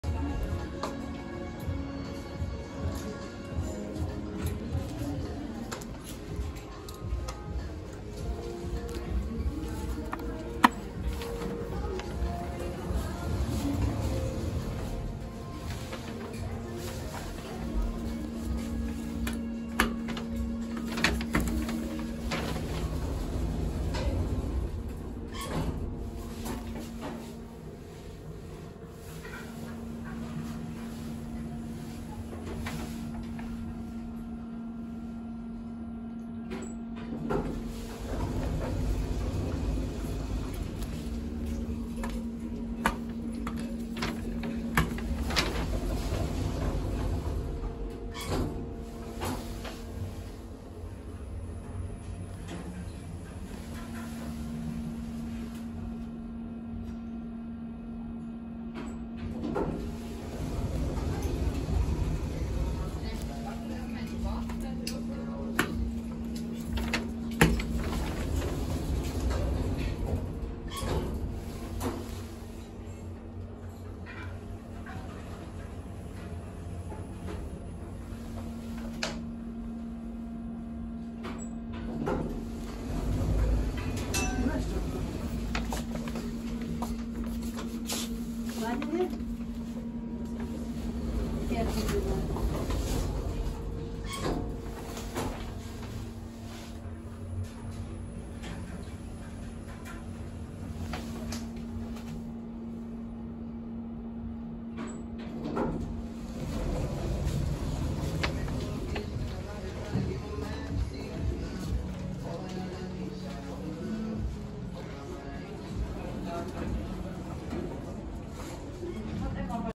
Otis Aufzug von 2018 am H&M - Kranzler Eck in Berlin
Normalerweise bin ich kein großer Fan von Aufzügen. Aber ich denke, das ist ein bisschen interessant. Also, warum nicht? Ein Otis Aufzug von 2018. Der Keller (Etage -1) ist geschlossen, da er nur für Mitarbeiter ist. Du wirst selbst sehen, woran das liegt. Ich hasse es wirklich, dass im Inneren ein beschissenen reflektierender Spiegel installiert ist. Sie müssen es wirklich loswerden!
Das Video wurde an dieser Adresse aufgenommen: Kurfürstendamm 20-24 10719 Berlin

© Video created by The Transit Channel
© The Transit Channel - Eine Weiterverwendung dieses Videos ist nicht gestattet. Jeglicher Verstoß gegen das Urheberrecht wird geahndet.